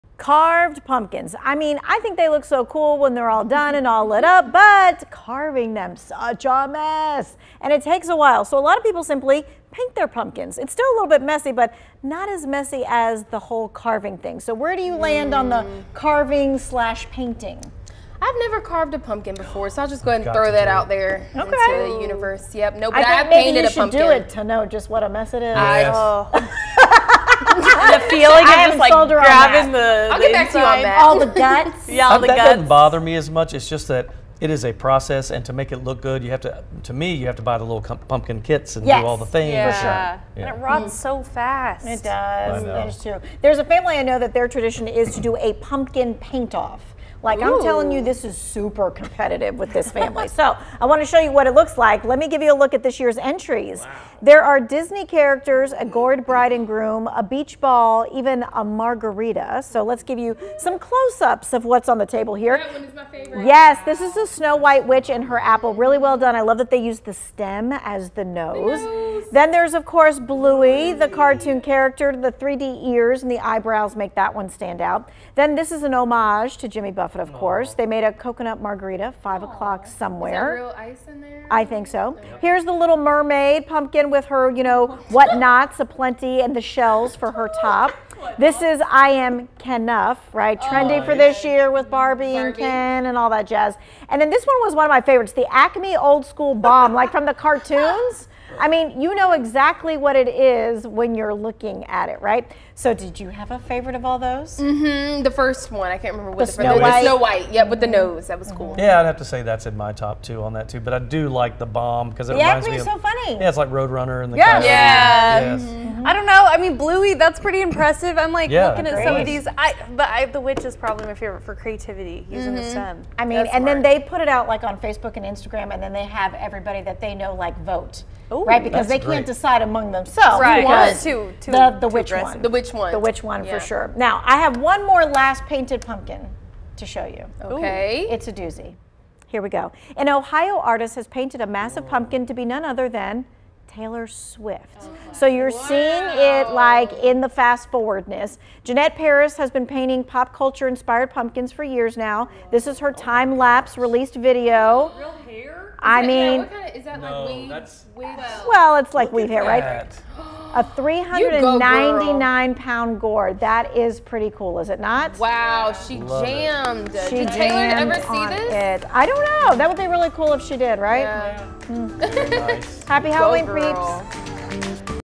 Why you should ditch the pumpkin carving tools for some paint brushes
Decorating for Halloween is a must and this year you can get creative by painting your pumpkins.